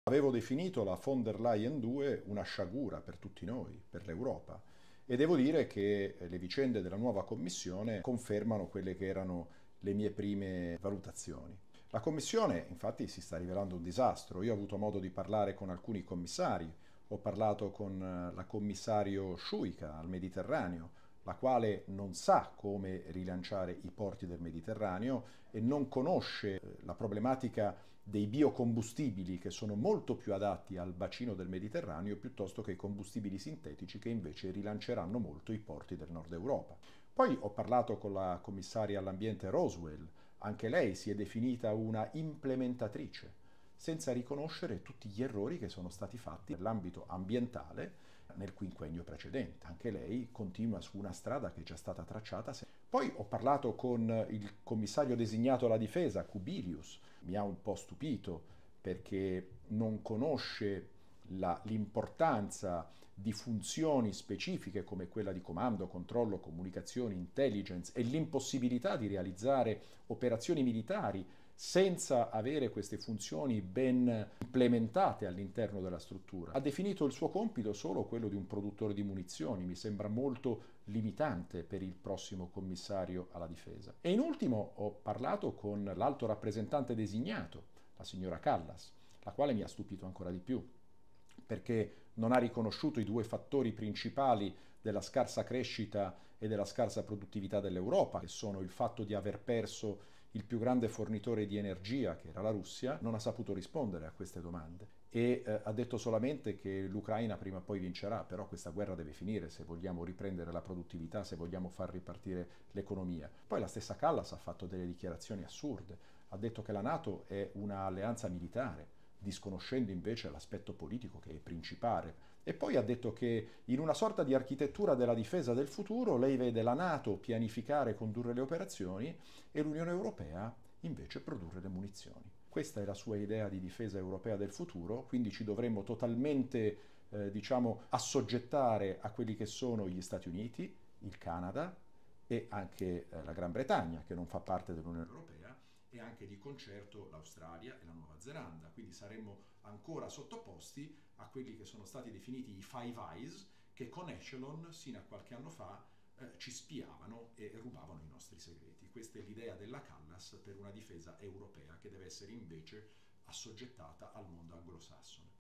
VANNACCI: "VON DER LEYEN 2, UNA SCIAGURA PER L'EUROPA"
(Agenzia Italia News) L'EUROPARLAMENTARE DELLA LEGA ROBERTO VANNACCI COMMENTA LA NUOVA COMMISSIONE EUROPEA